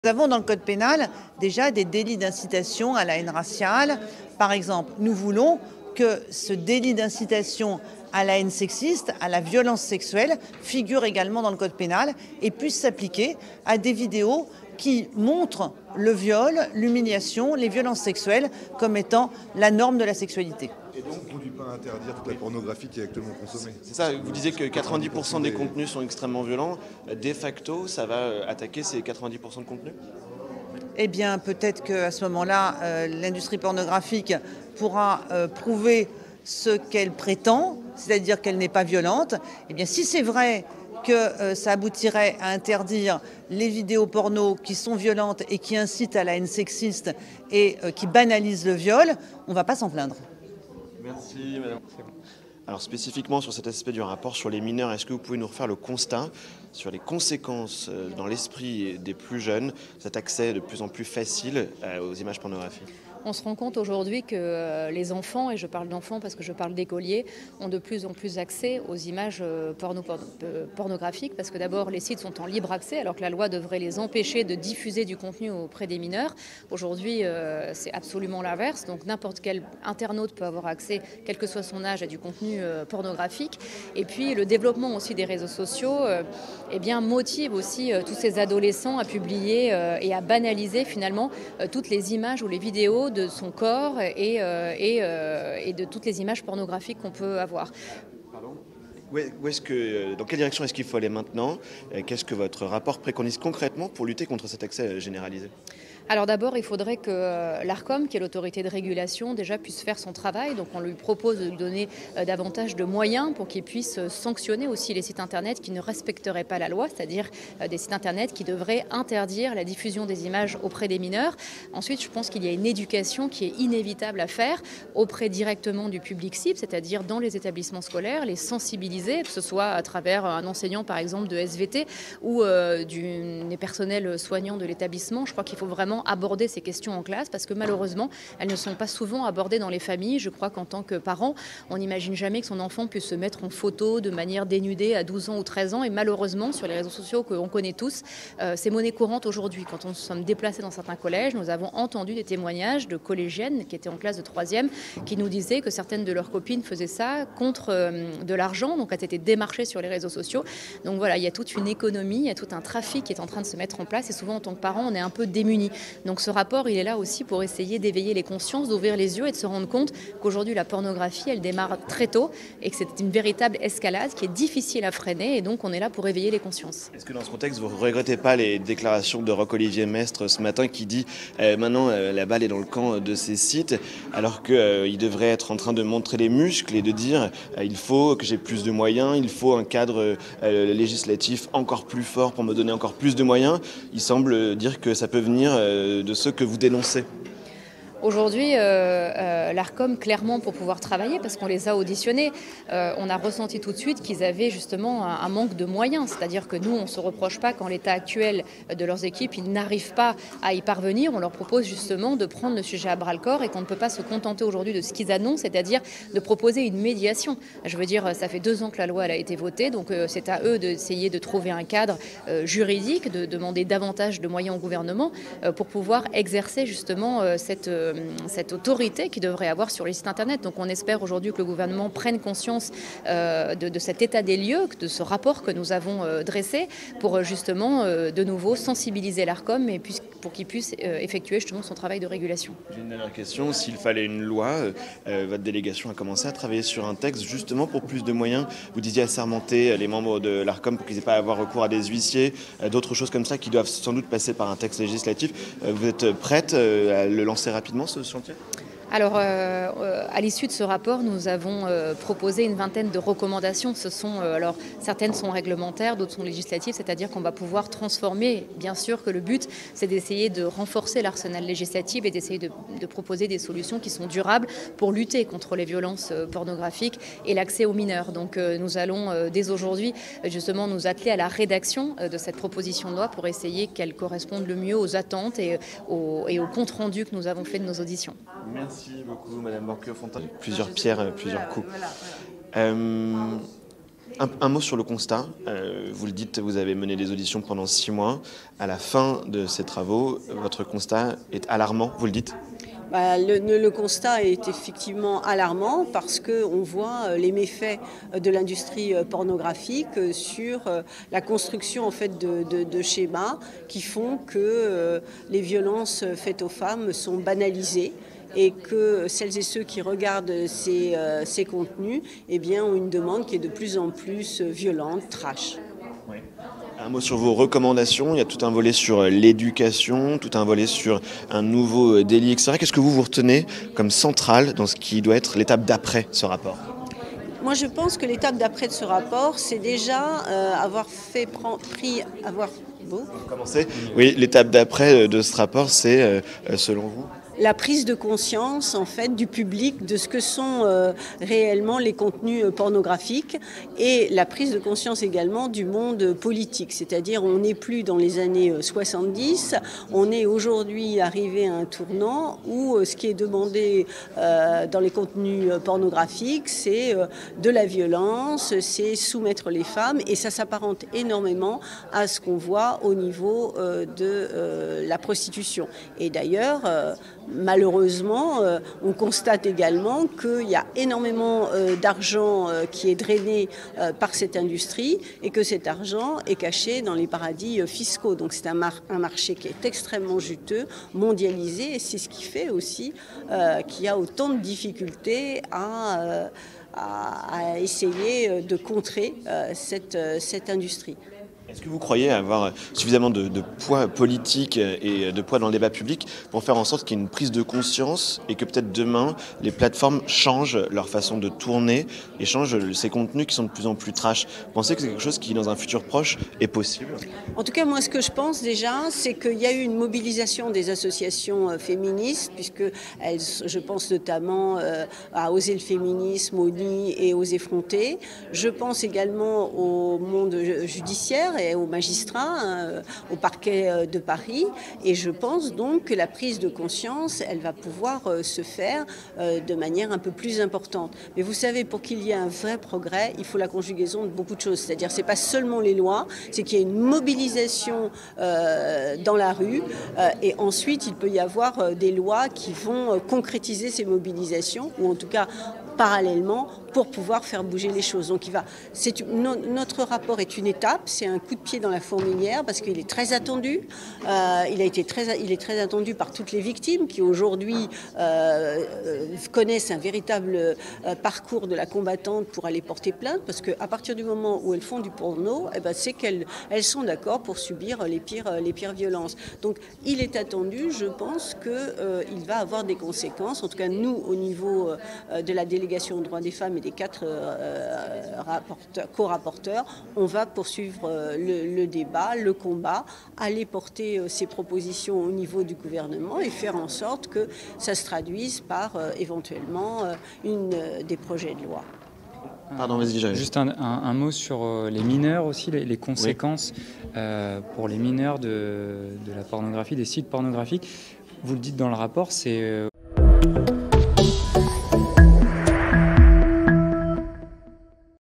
Porno : la mission du Sénat veut créer un délit d’incitation aux violences sexuelles
C’est l’un des points forts des propositions de la mission d’information du Sénat sur l’industrie pornographique. Faire des violences sexuelles commises dans un contexte de pornographe, un délit d’incitation à une infraction pénale. "Et si ça aboutit à interdire les vidéos pornos qui sont violentes qui aboutissent à la haine sexiste et qui banalisent le viol, on ne va pas s’en plaindre", souligne Laurence Rossignol, co-rapporteure du rapport parlementaire.